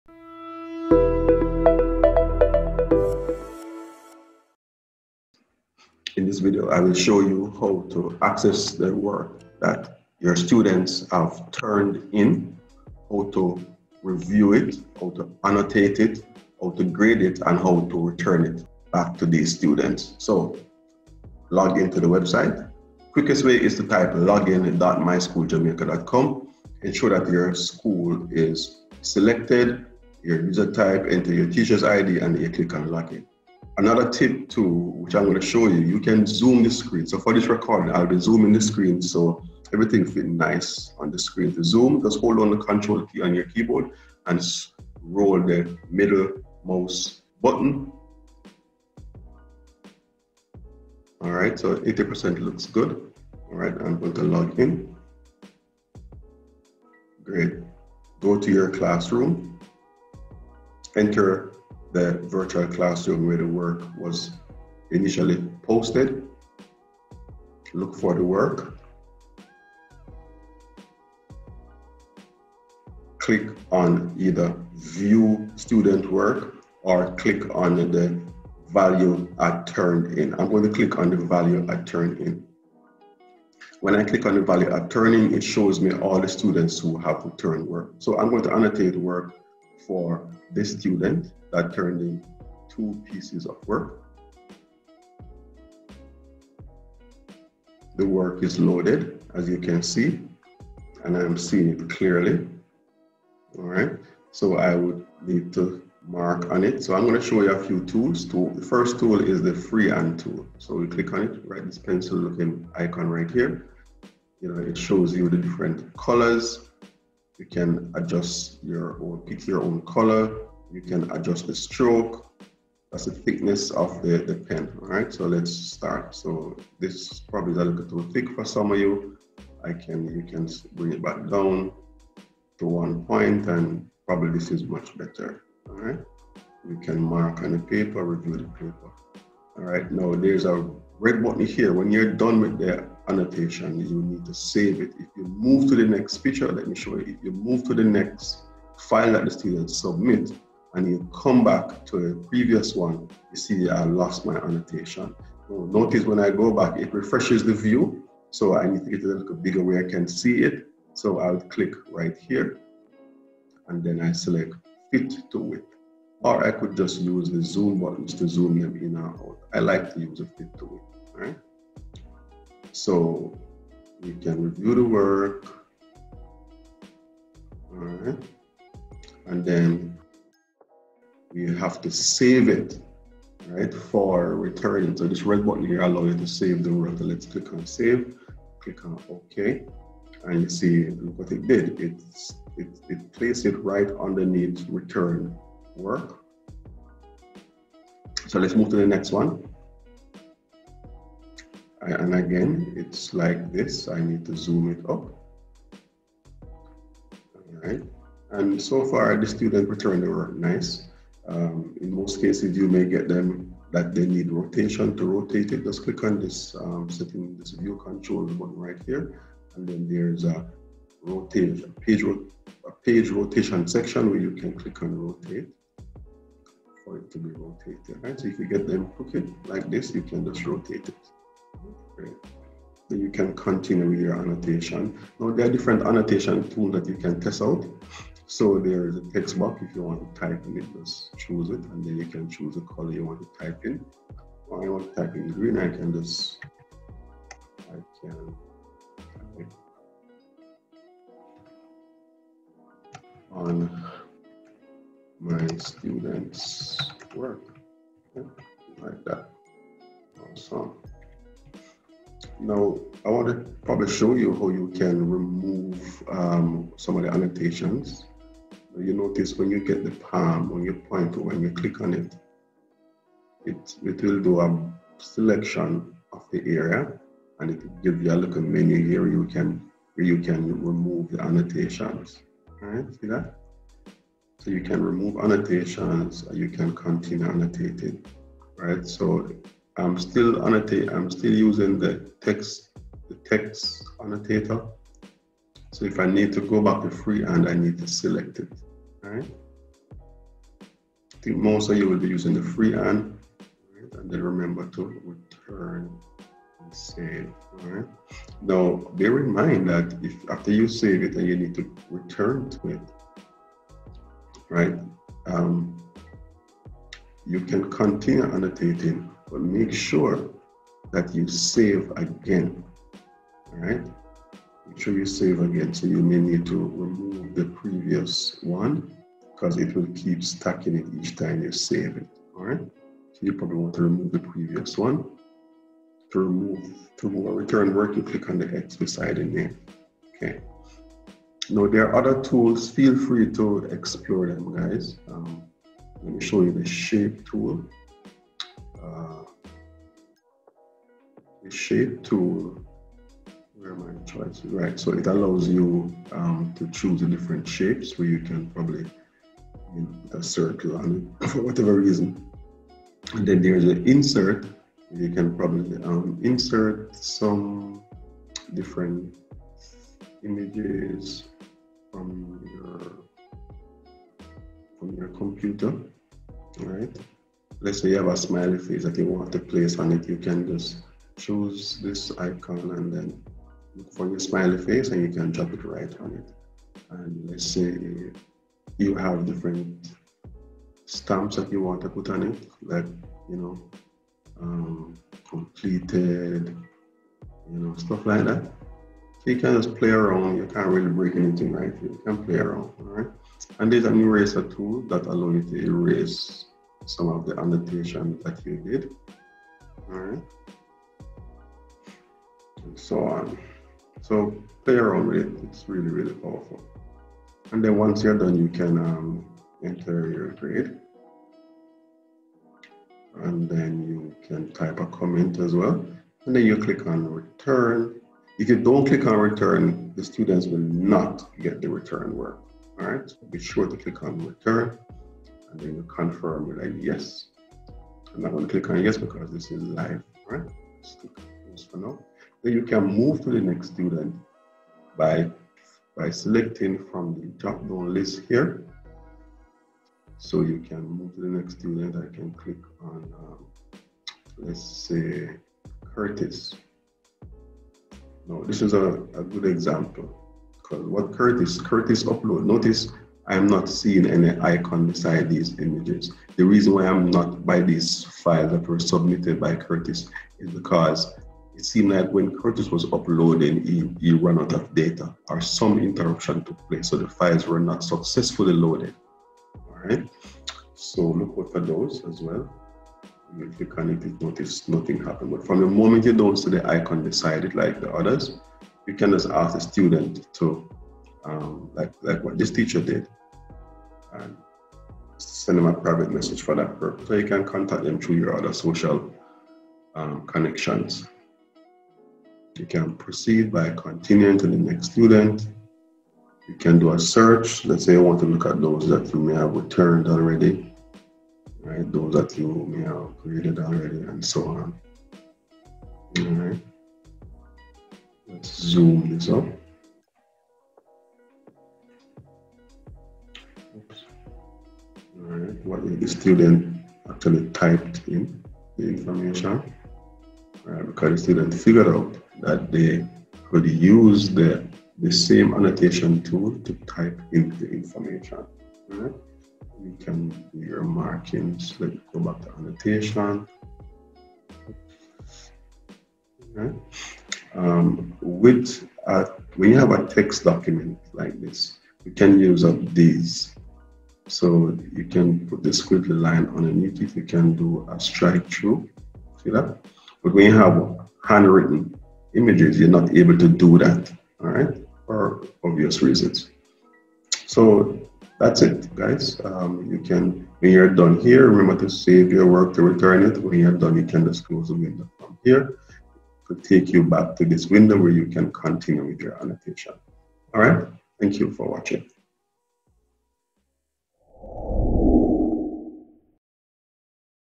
How to access, review, annotate, grade and return student's work in MySchool LMS
The official Channel for Comsol Jamaica
In this video you will learn how to access, review, annotate, grade and return student's work in MySchool LMS